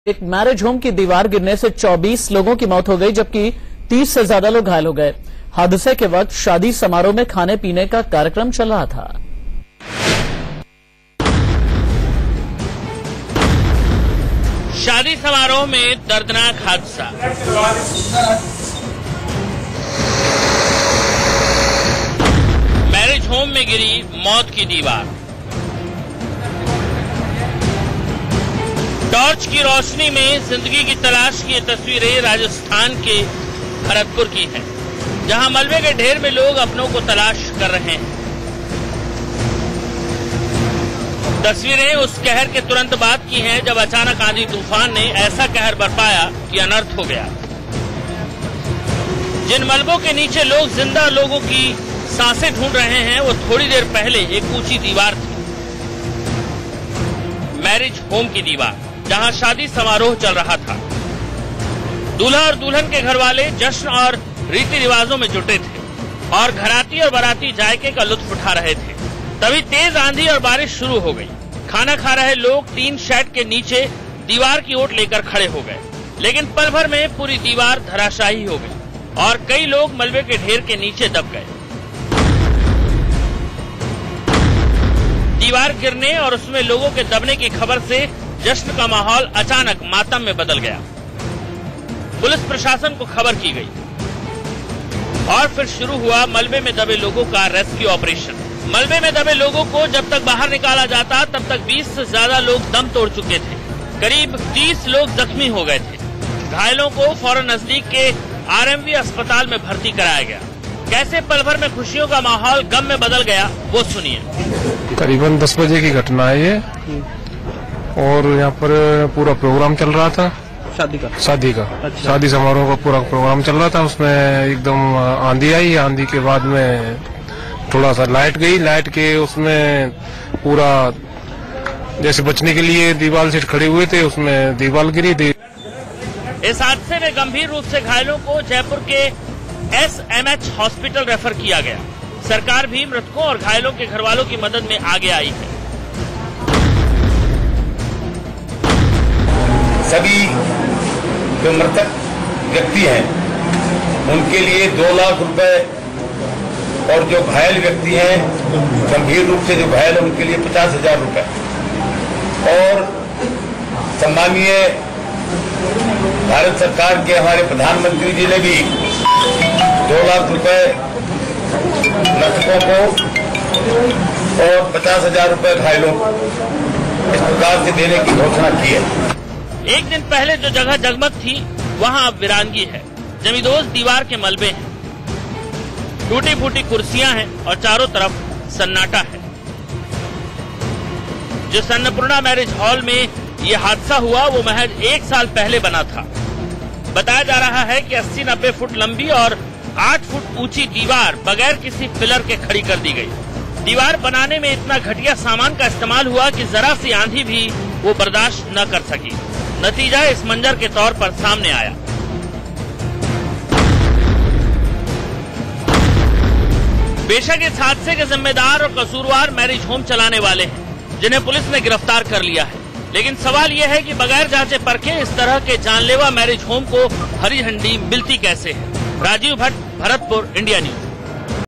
Bharatpur wall collapse: 24 dead, 30 injured at wedding hall in Rajasthan
24 Dead and 30 injured after wall of a wedding hall collapses in Rajasthan's Bharatpur.